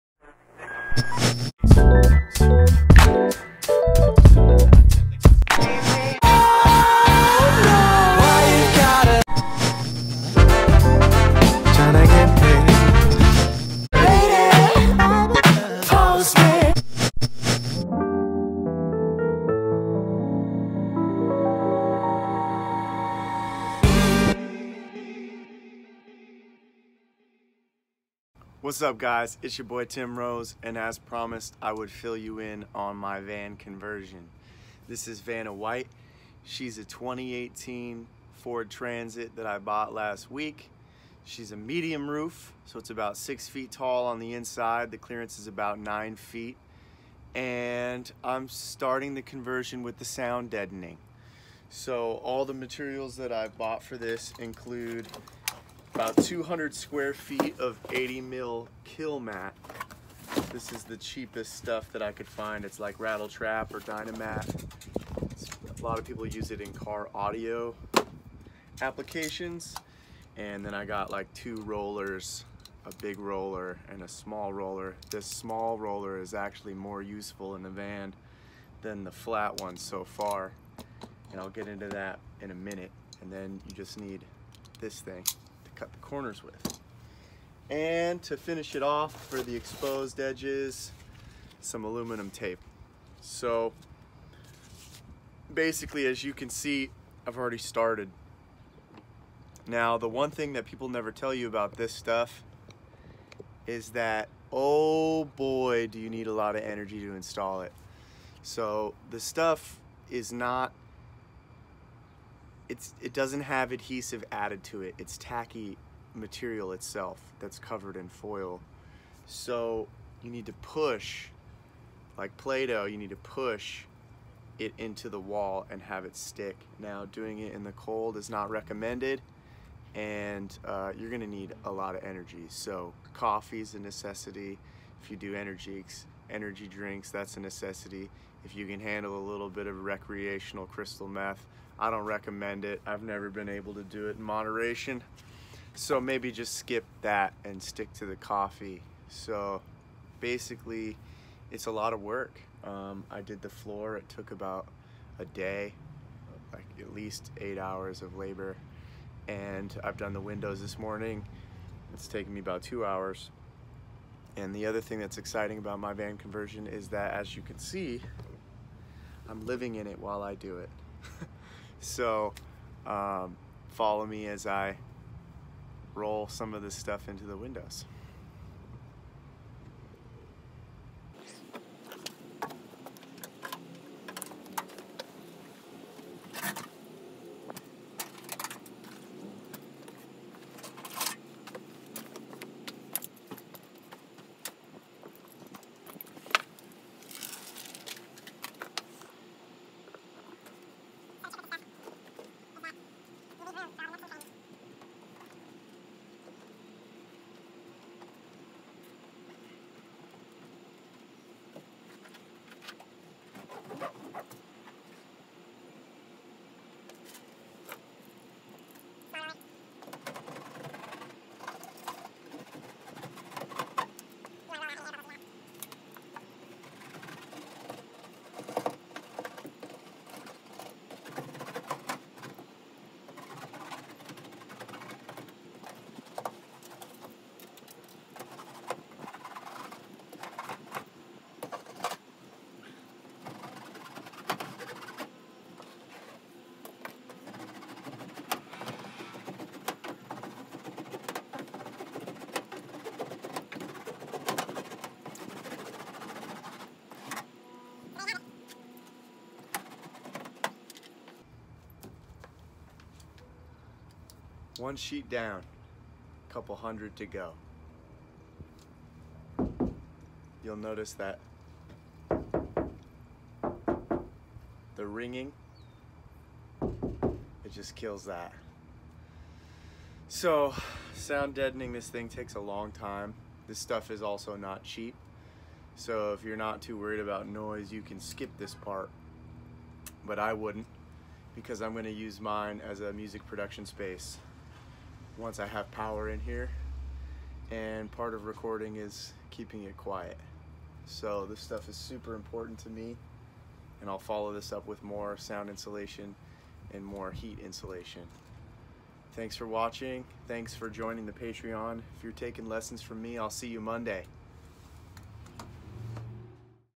Sound Deadening the back of a Ford Transit
Materials Needed:
Kilmat 80mil Butyl Sheets
Aluminum Tape
A ton of energy